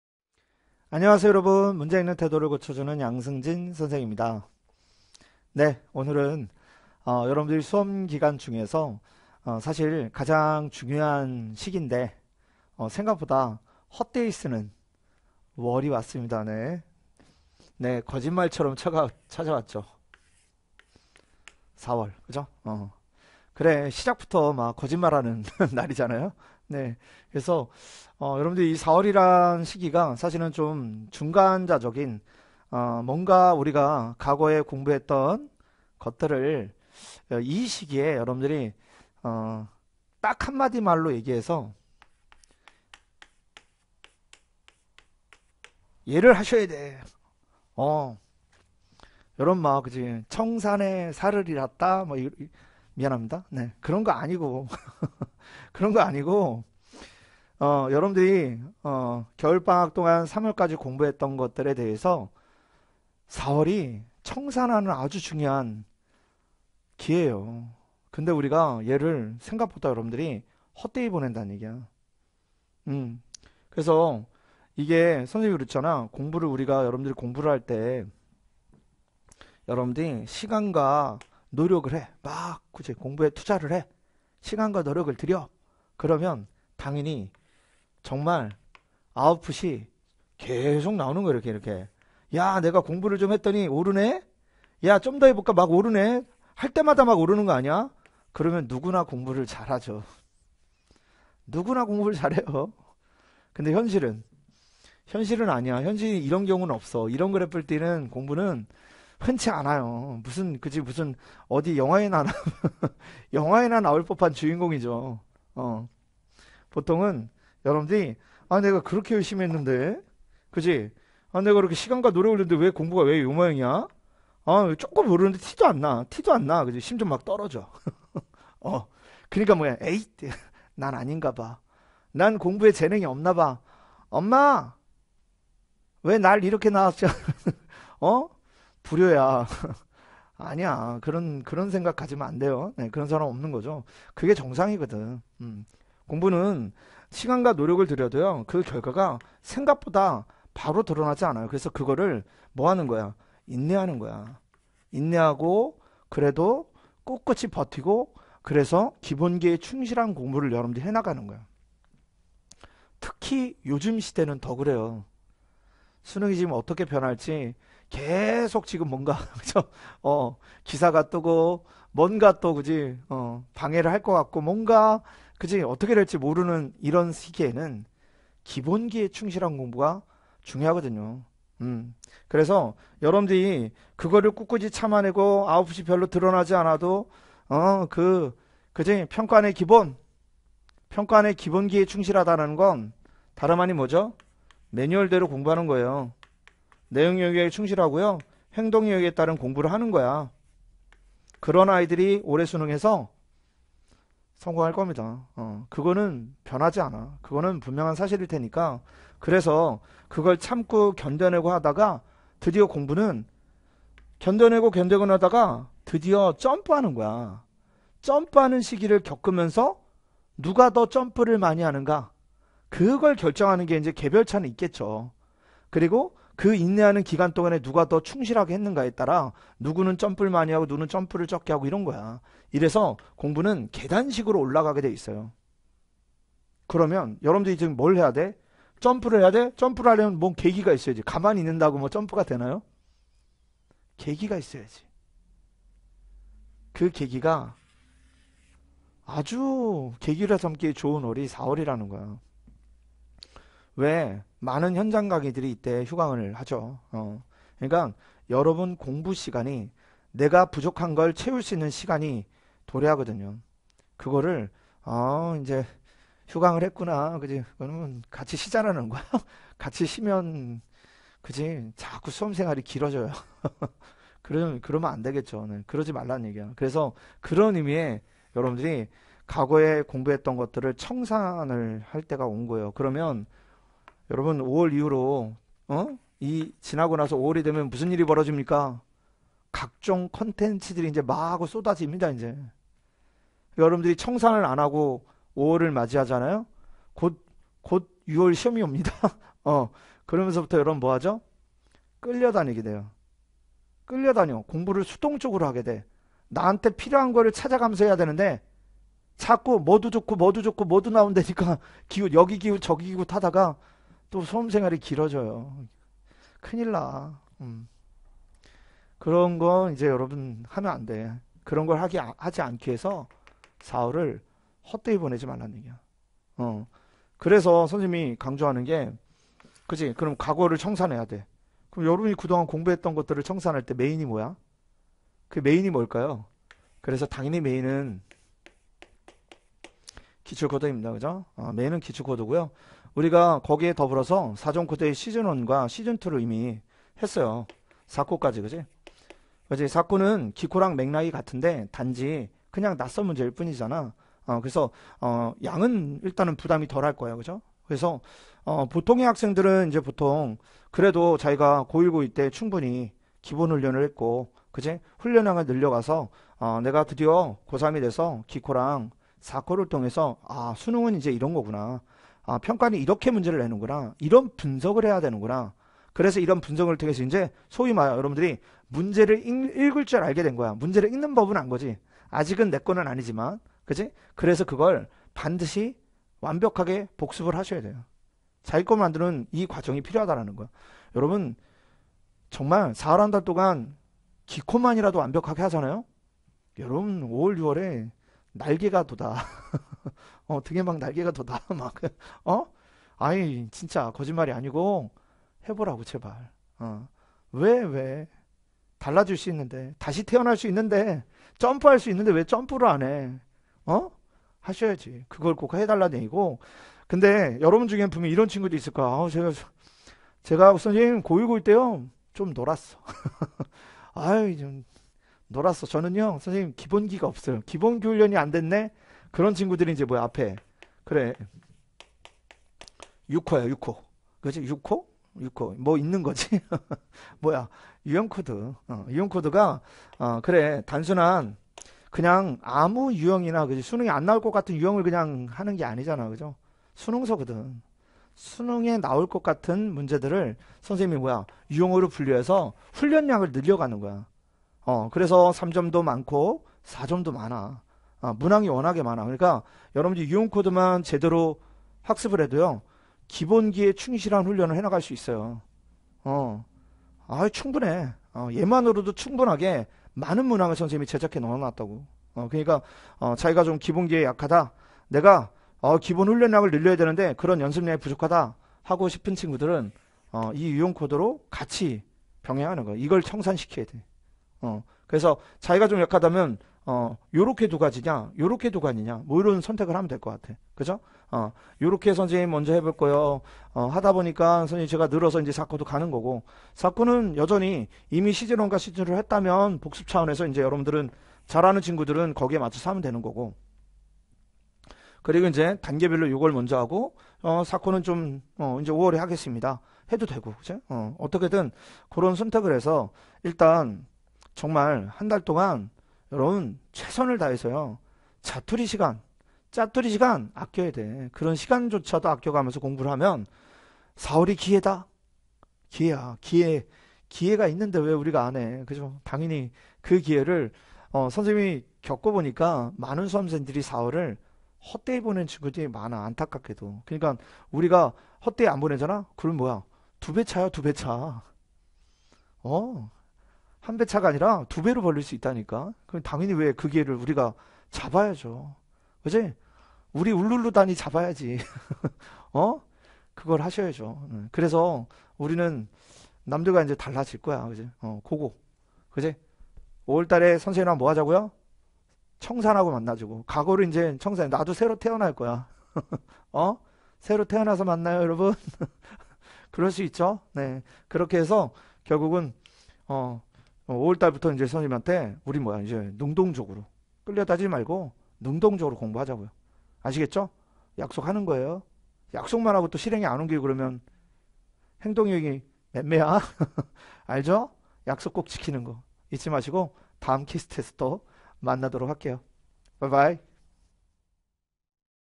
[메가스터디] 수학 양승진쌤 - 청산
4월은 겨울 방학부터 3월까지 배웠던 것들을 청산하는 중요한 기회입니다!

공부는 시간과 노력을 들여도 그 결과가 바로 드러나지 않아서
인내하고 꿋꿋이 버티면서 기본기에 충실한 공부를 해나가야 합니다:)

계단식으로 오르는 실력을 점프하는 시기인 4월을 헛되이 보내면 안 됩니다

자세한 내용은 영상을 통해 확인해 주세요 ٩꒰｡•◡•｡꒱۶

- 수학 양승진 선생님을 더 알고 싶다면?